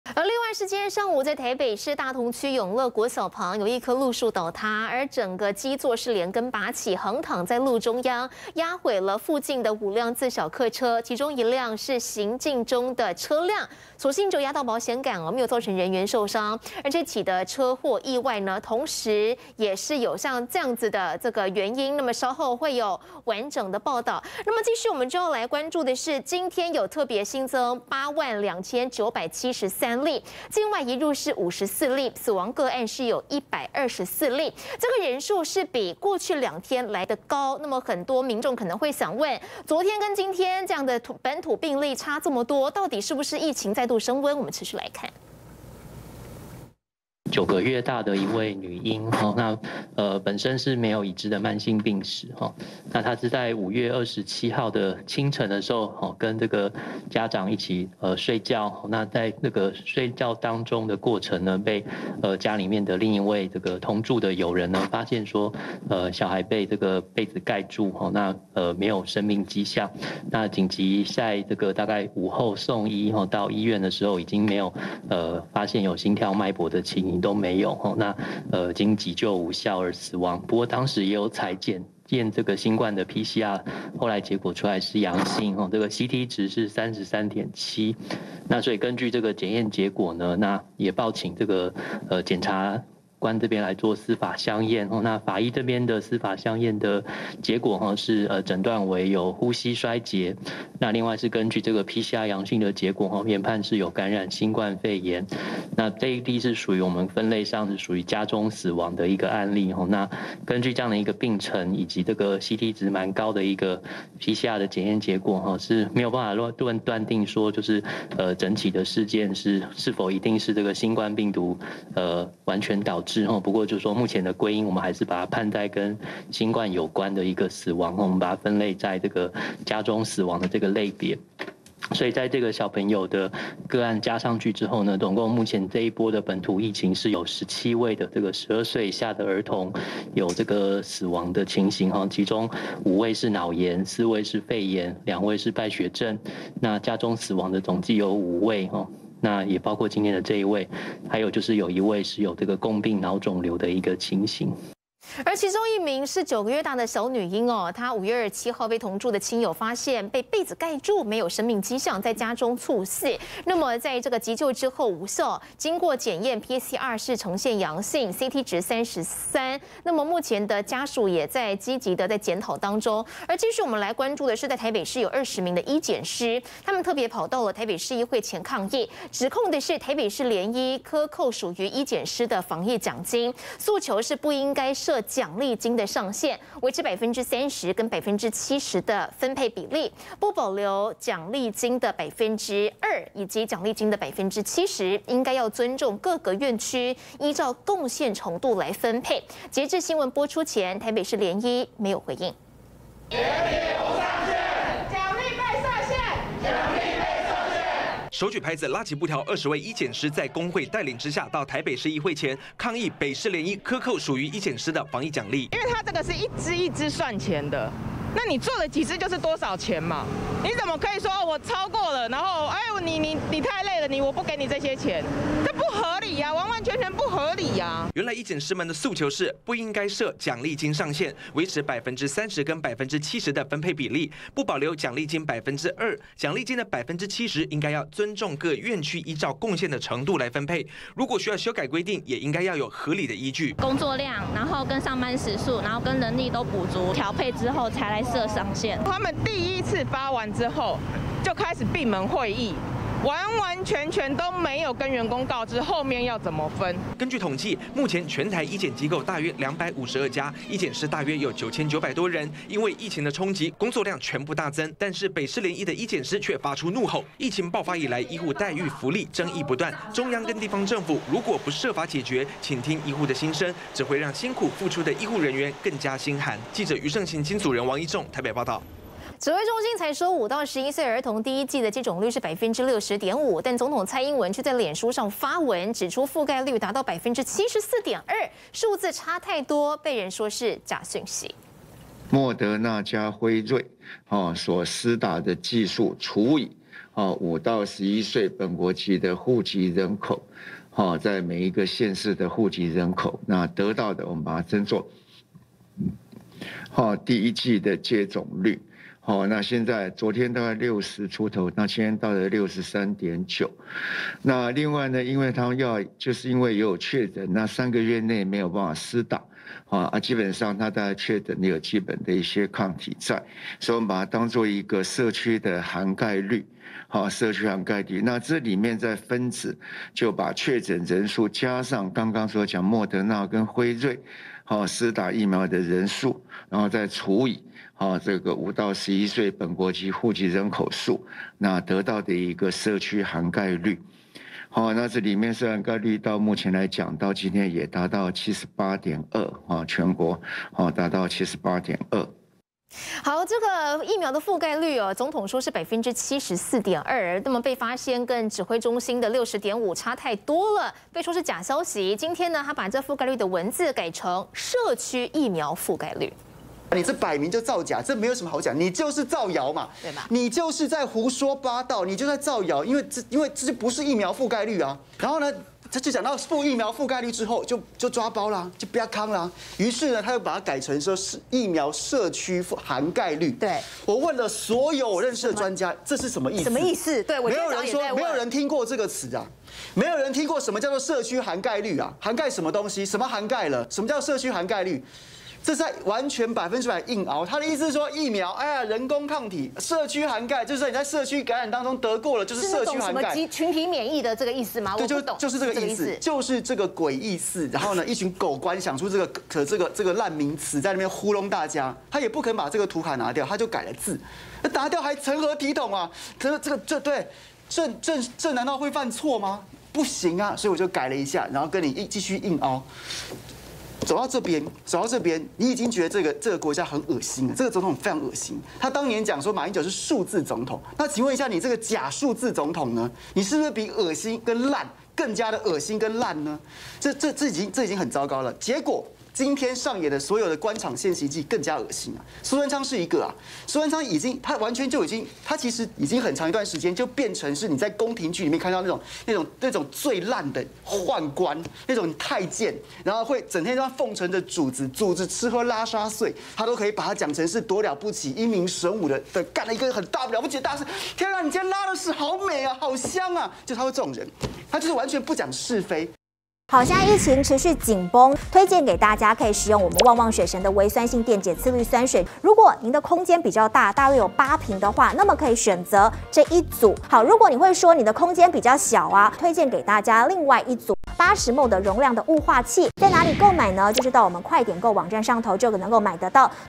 【每日必看】蔡灌水疫苗覆蓋率? 臉書"換圖"創新名詞?｜今本土+8萬2973例 高雄.台中躍升前兩名@中天新聞CtiNews 20220607
00:24 今本土+8萬2973例 高雄.台中躍升前兩名
02:35 再增1嬰兒"家中猝逝" 9個月大女嬰死後確診
02:53 醫檢師抗議! 怒控北市聯醫"苛扣防疫獎勵金"
04:39 蔡稱5-11歲"覆蓋率"74.2% 挨轟"大內宣"
05:53 蔡灌水疫苗覆蓋率? 臉書"換圖"創新名詞?